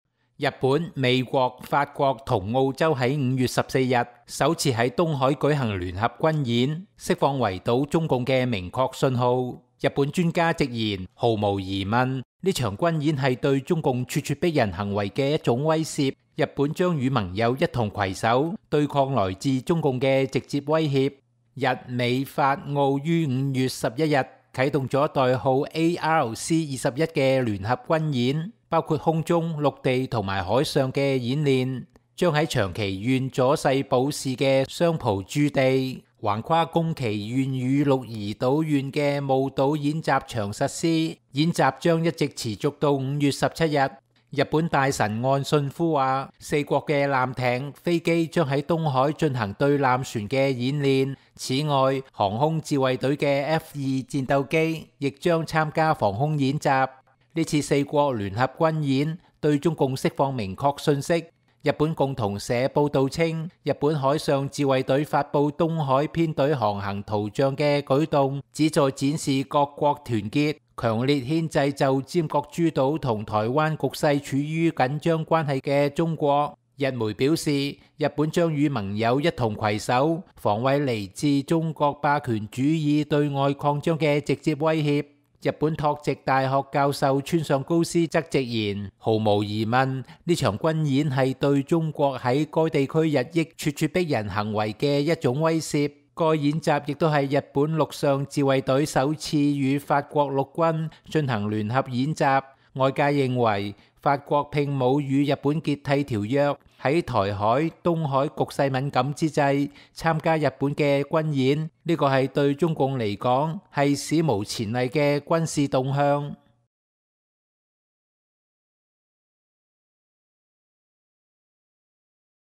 (字幕) 日美法澳近日首次在東海舉行聯合軍演，釋放圍堵中共的明確信號。日本專家直言，毫無疑問，這場軍演是對中共咄咄逼人行為的一種威懾。
日美法澳 東海舉行聯合軍演 圍堵中共#香港大紀元

請支持大紀元：
📍贊助大紀元：